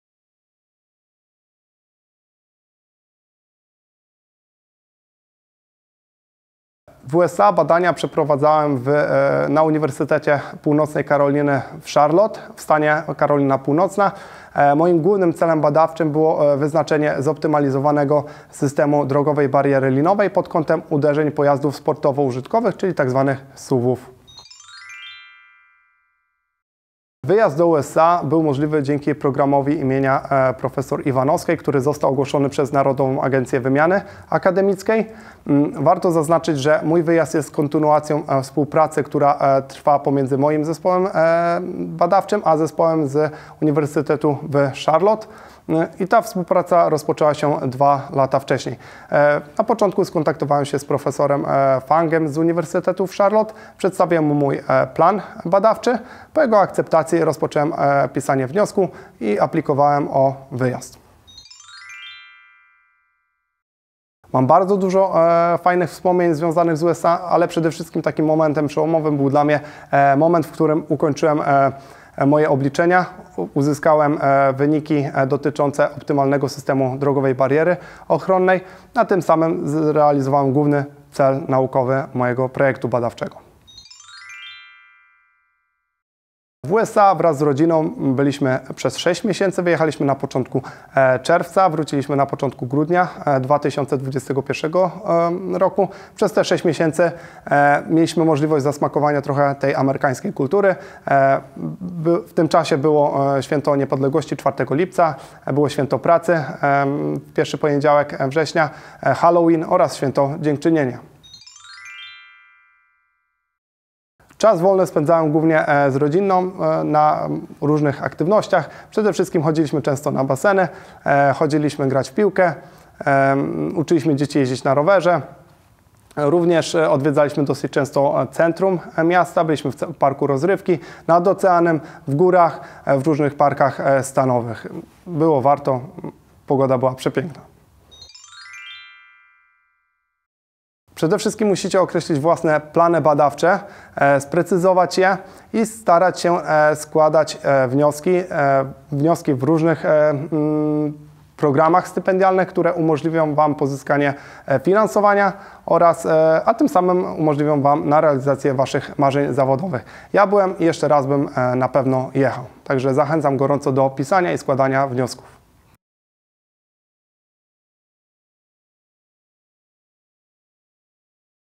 USA - STUDIA
Jak wyjechać na zagraniczny staż naukowy?

Pan dr inż. Dawid Bruski z Katedry Wytrzymałości Materiałów zgodził się opowiedzieć o swoich doświadczeniach związanych z wyjazdem na staż naukowy do USA. Zachęcamy do obejrzenia filmu!

Cykl POZNAJ NAS LEPIEJ to krótkie historie opowiedziane z pasją przez pracowników i studentów Wydziału Inżynierii Lądowej i Środowiska Politechniki Gdańskiej.